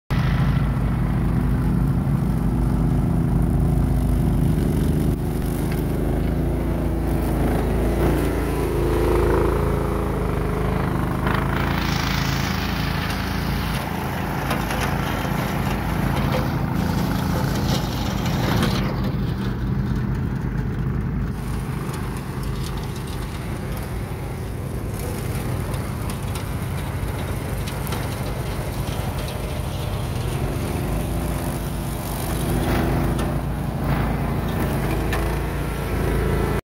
Kosiarka karczownica orec RM830 wyjazd z krzaków
Wjechanie w bardzo geste zarośla nie spowodowało zatrzymania pracy silnika wiec maszyna do zadań specjalnych zdaje egzamin :)